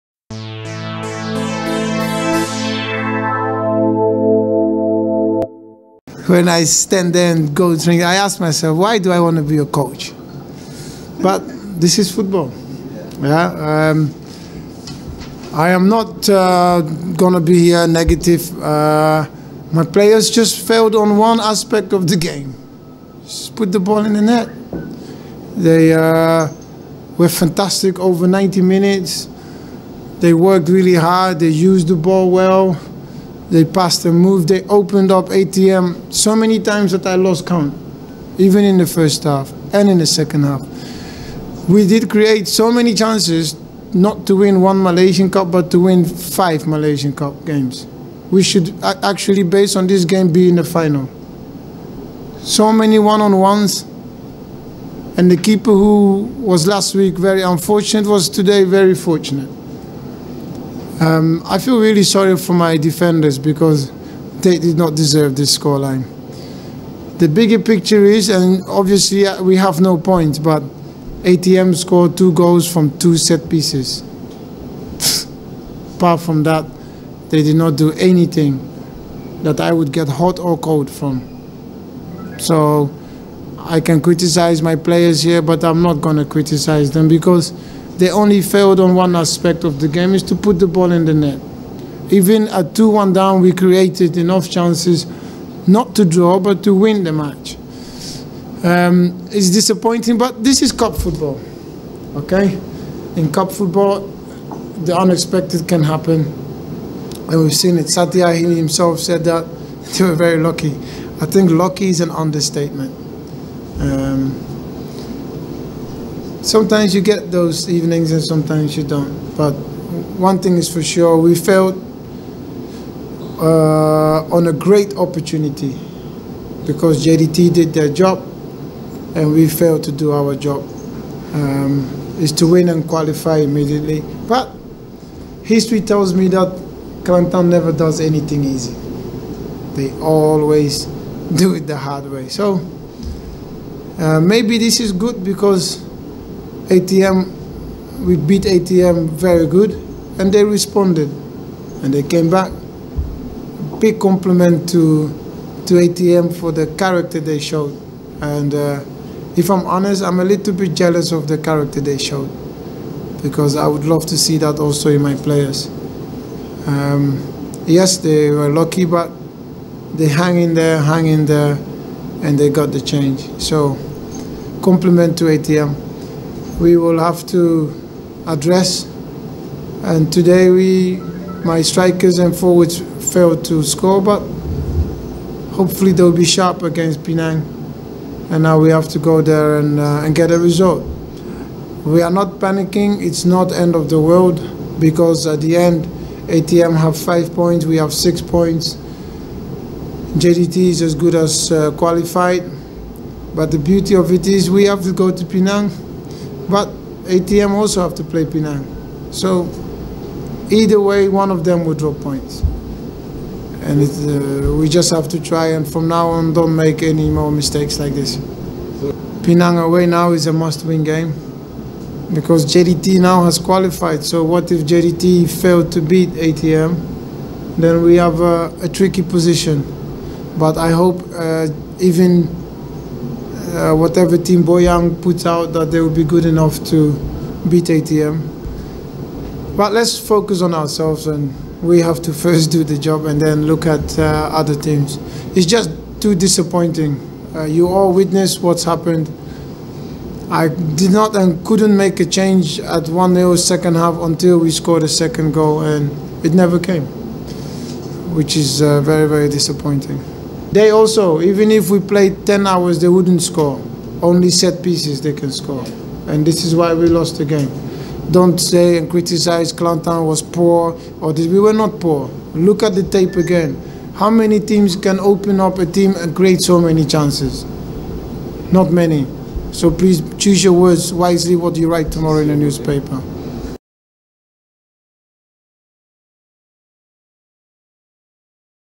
26.8.14 Sidang Media bersama George Boateng
Temuramah selepas perlawanan pada 26 Ogos yang menyaksikan sekali lagi ATM berjaya menewaskan Kelantan di gelanggang sendiri.

Piala Malaysia Peringkat Kumpulan Pusingan ke 2
Kelantan 1:2 ATM